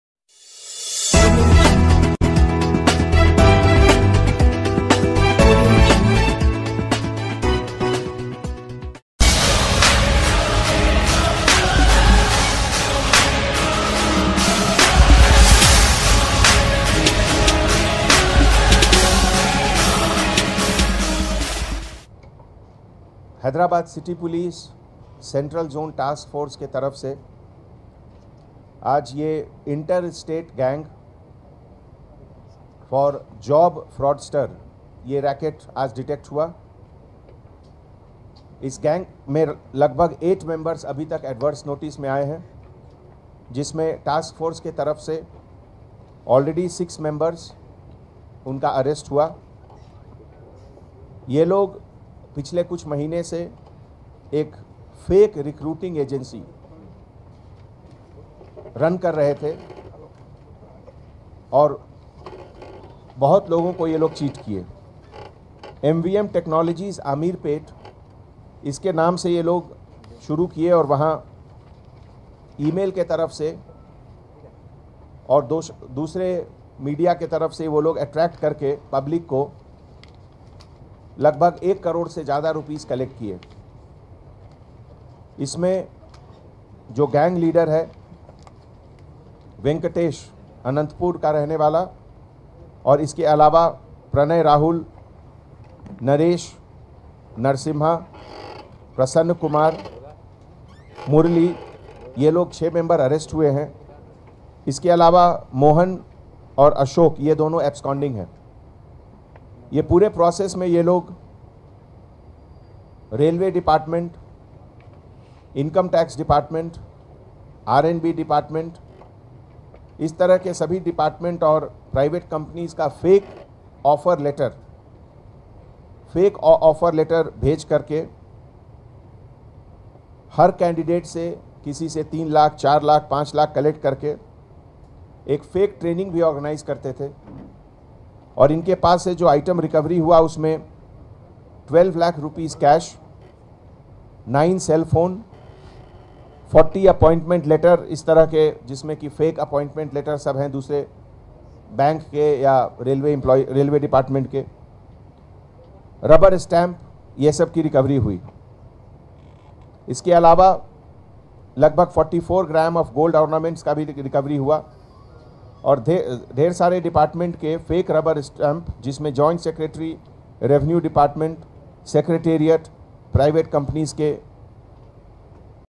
Inter State Gang of Job Fraudsters Racket arrested Central zone task force police CP Hyd
Central zone Task Force Team Apprehended one gang of 6 persons arrested who cheated unemployed youth for providing jobs in SBI, IT, R&B, IRRIGATION, RAILWAY and collected about 2 CRORES and issued fake appointment letters fake joining orders , seized fake orders, rubber stamps, laptop Cash of 12 Lakhs, & gold etc.